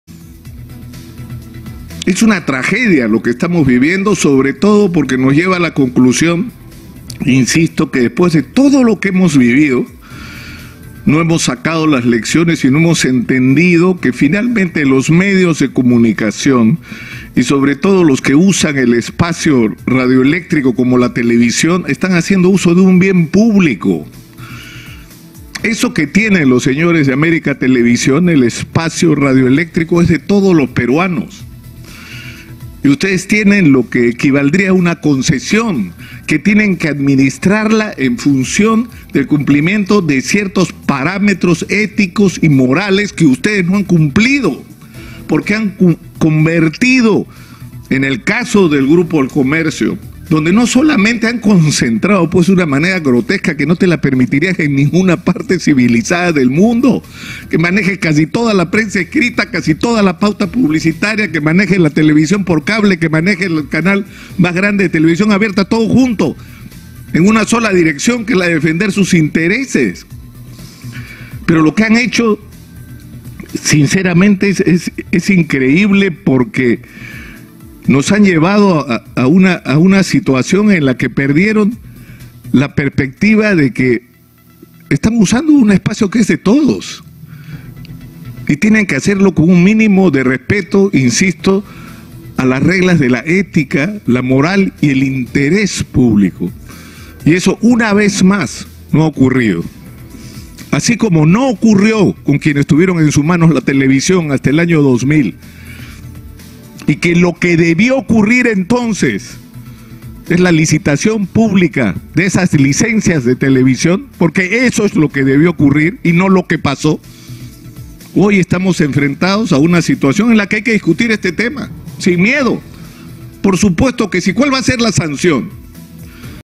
NICOLAS LUCAR: AMÉRICA TELEVISIÓN ESTÁ HACIENDO MAL USO DEL ESPACIO RADIOELÉCTRICO DE LOS PERUANOS.
El periodista NICOLAS LUCAR denunció que AMÉRICA TELEVISIÓN ESTÁ HACIENDO MAL USO DEL ESPACIO RADIOELÉCTRICO DE LOS PERUANOS y le están faltando el respeto a los peruanos.